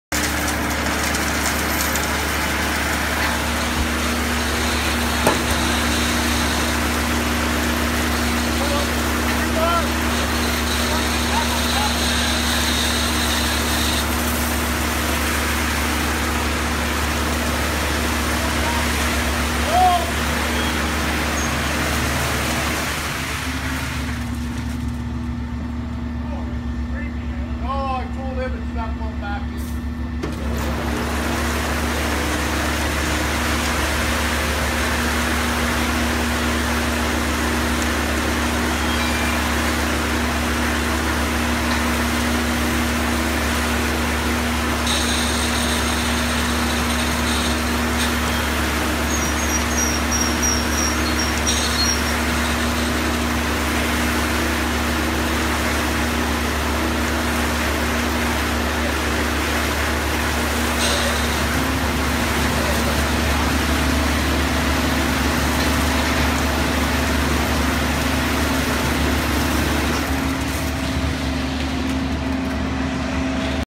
Gravel Delivery part 2.
So after pouring the original floor, I decided the shop was not going to be big enough.  So, I applied to the city and got approval to add 9' to it.